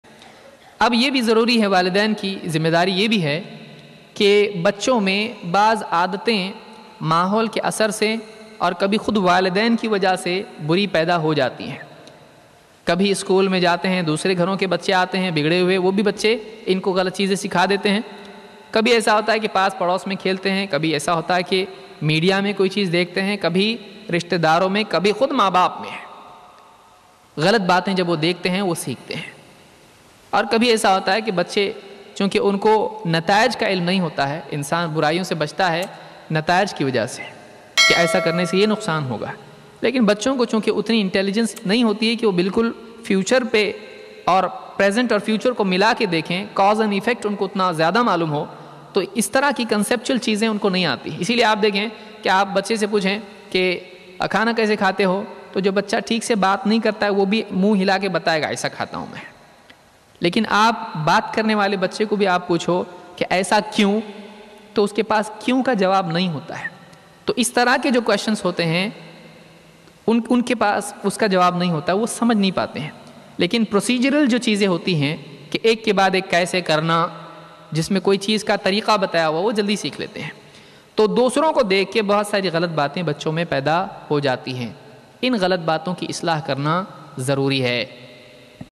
Dusroun ko dekh kar Bache bigad jate hain | Shaikh Abu Zaid Zameer
This video clips is taken from Aulad Ki Tarbiyat Me Maa Baap Ka Kirdaar by Shaikh Abu Zaid Zameer